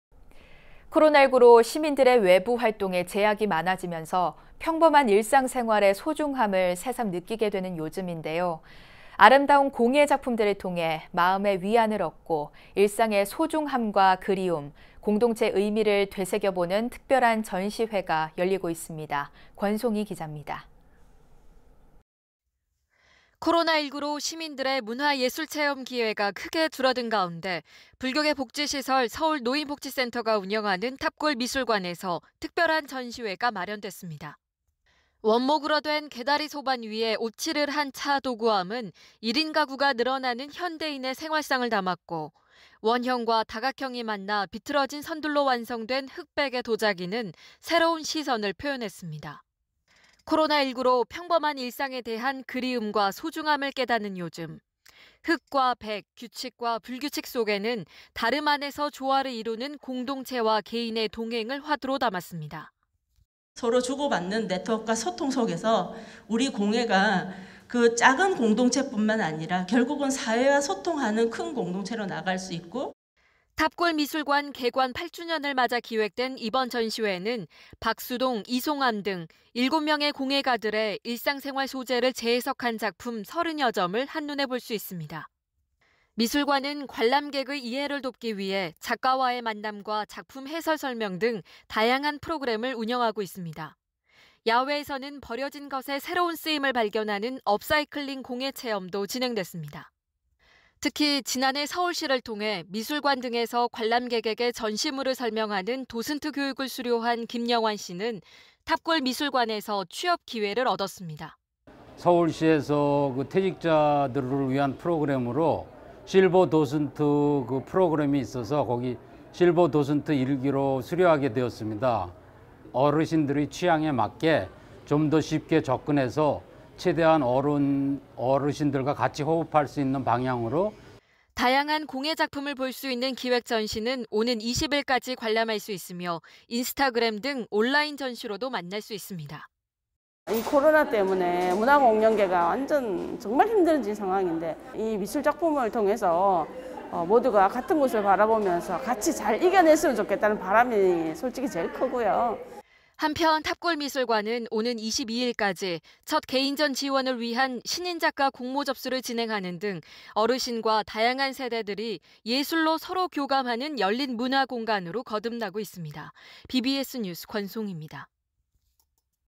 [BBS뉴스] '일상의 소중함을 전해요'...탑골 미술관 공예동행전
코로나19로 시민들의 외부 활동에 제약이 많아지면서 평범한 일상 생활의 소중함을 새삼 느끼게 되는 요즘인데요.

아름다운 공예 작품들을 통해 마음에 위안을 얻고 일상의 소중함과 그리움, 공동체의 의미를 되새겨보는 특별한 전시회가 열리고 있습니다.

권송희 기자입니다.